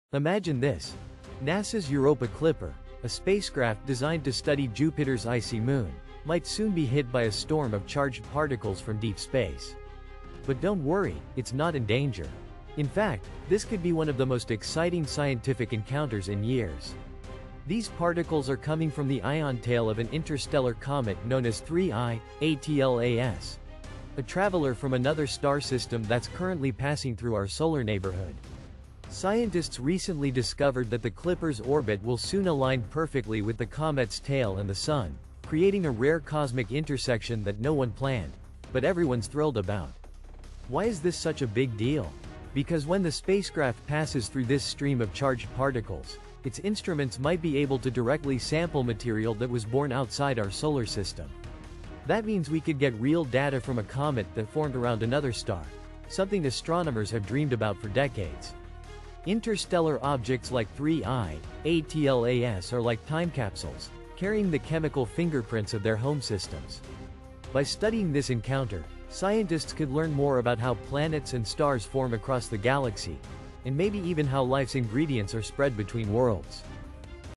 🚀✨ NASA’s Europa Clipper to Encounter Interstellar Comet!
Imagine this — NASA’s Europa Clipper, a spacecraft designed to study Jupiter’s icy moon, might soon be hit by a storm of charged particles from deep space. But don’t worry — it’s not in danger. In fact, this could be one of the most exciting scientific encounters in years. These particles are coming from the ion tail of an interstellar comet known as 3I/ATLAS — a traveler from another star system that’s currently passing through our solar neighborhood. Scientists recently discovered that the Clipper’s orbit will soon align perfectly with the comet’s tail and the Sun, creating a rare cosmic intersection that no one planned — but everyone’s thrilled about. Why is this such a big deal? Because when the spacecraft passes through this stream of charged particles, its instruments might be able to directly sample material that was born outside our solar system. That means we could get real data from a comet that formed around another star — something astronomers have dreamed about for decades. Interstellar objects like 3I/ATLAS are like time capsules, carrying the chemical fingerprints of their home systems. By studying this encounter, scientists could learn more about how planets and stars form across the galaxy — and maybe even how life’s ingredients are spread between worlds. 🌌🛰️🔬